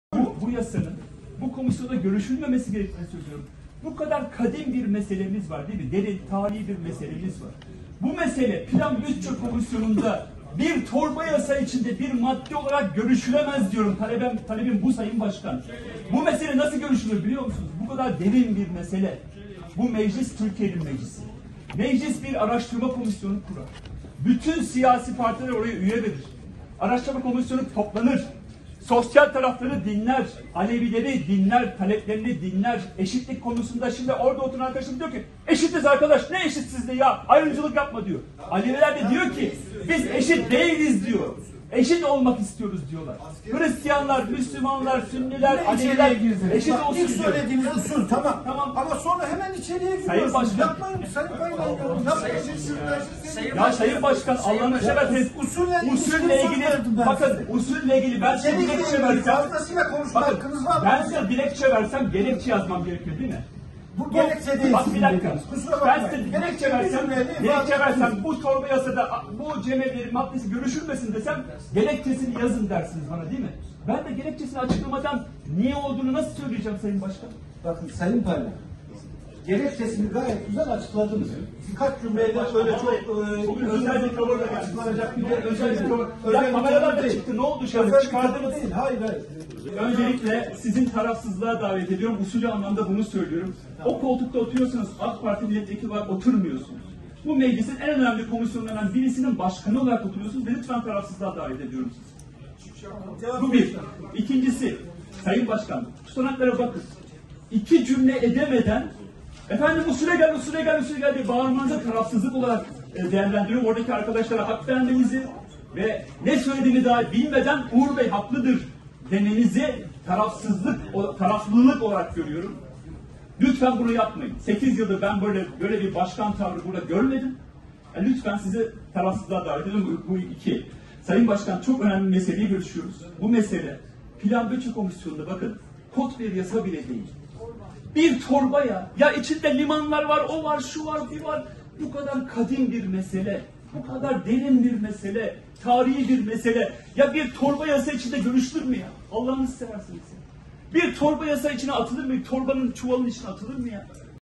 'Cemevleri torba yasa içinde görüşülemez' diyen Paylan'a 'Sen müslüman değilsin' çıkışı-VİDEO
PİRHA – TBMM Plan ve Bütçe Komisyonu'nda ele alınan torba yasada Alevileri de ilgilendiren başlıkların bulunması üzerine Milletvekili Garo Paylan’dan itiraz geldi. “Bu mesele bu komisyonda bir madde olarak görüşülemez” diyen Paylan’a AKP sıralarından “Sen Müslüman değilsin, Alevilerden sana ne” çıkışı geldi.